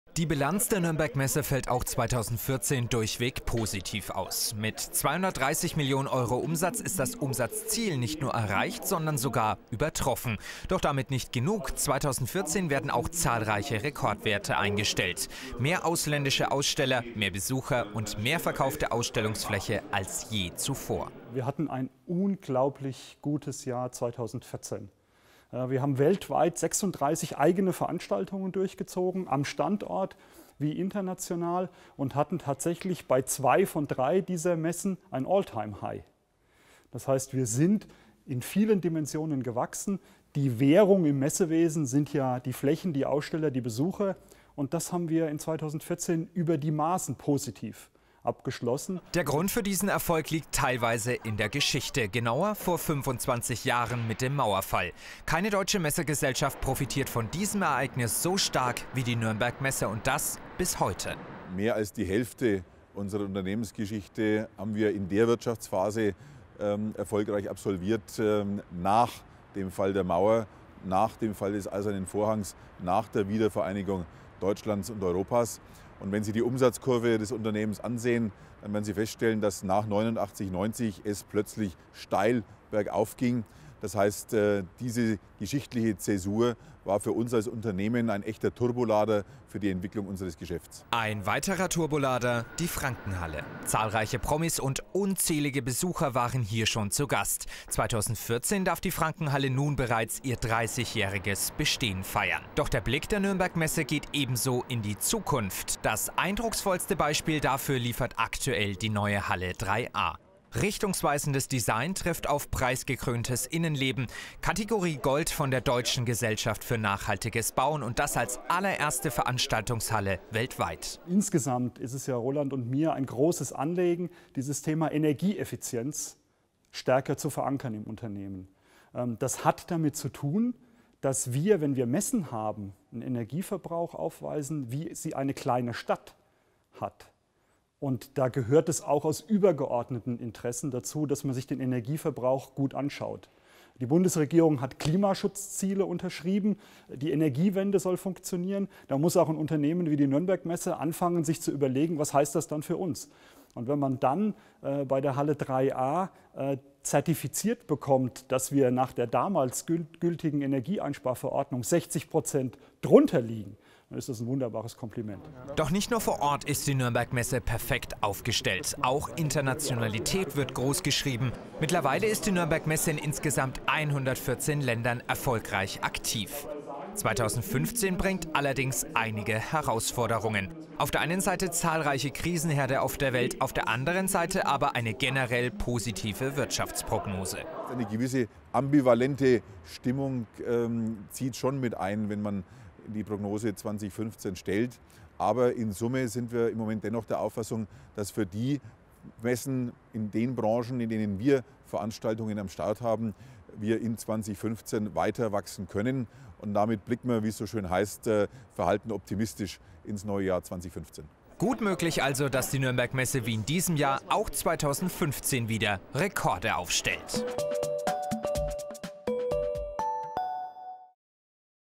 Jahrespressekonferenz der NürnbergMesse: Gewinner der Wiedervereinigung
Die CEOs der NürnbergMesse Group, Dr. Roland Fleck und Peter Ottmann, zu 25 Jahre Mauerfall und dem Rückblick auf das Geschäftsjahr 2014